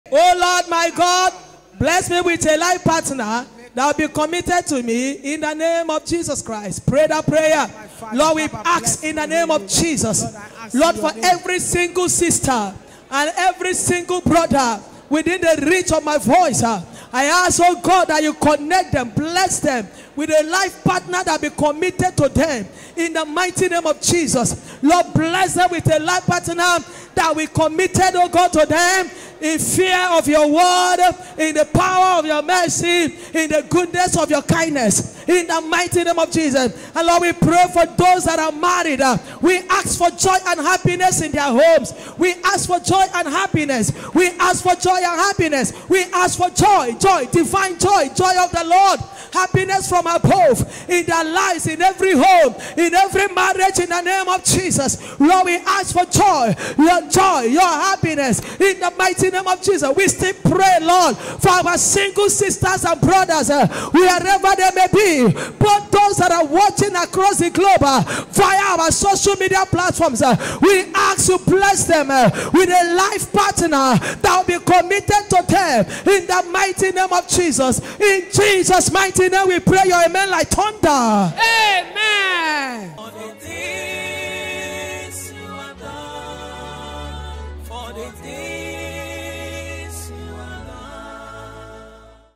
Prayer for a Life Partner from God | Pray with Prophetess Chidinma Mbanefo
Prayer for a Life Partner from God